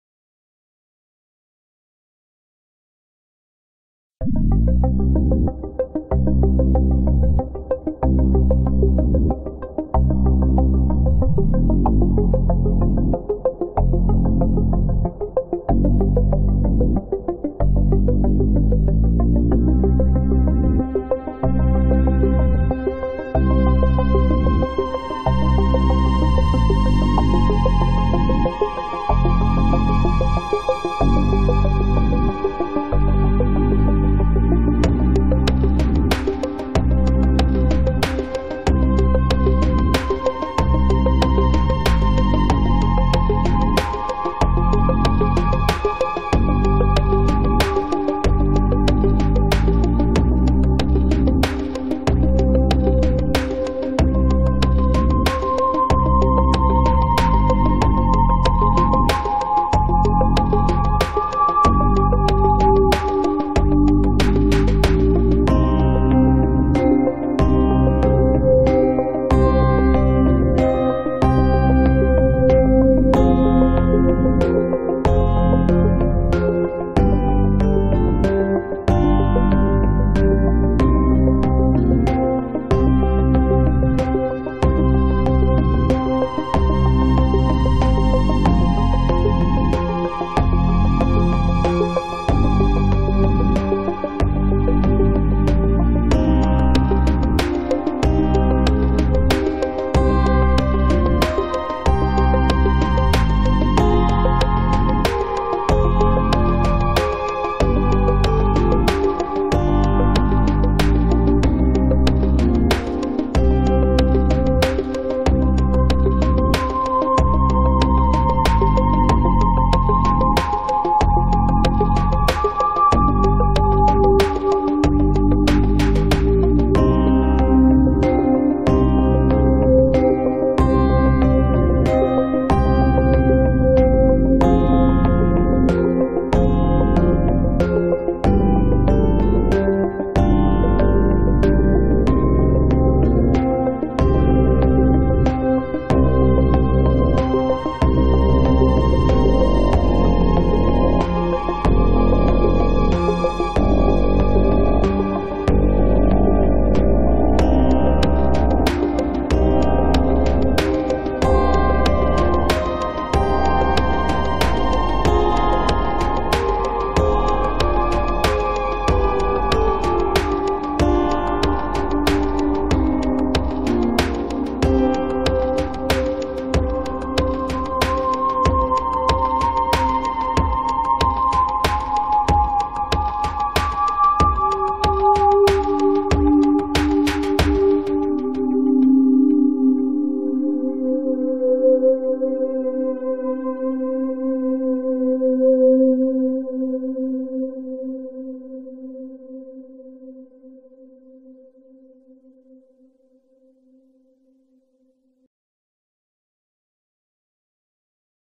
Ersticktes Brennen
Ein weiterer langsamer Zoom in das Burning-Ship-Fraktal zu monotoner und hochgradig repetitiver Computermusik.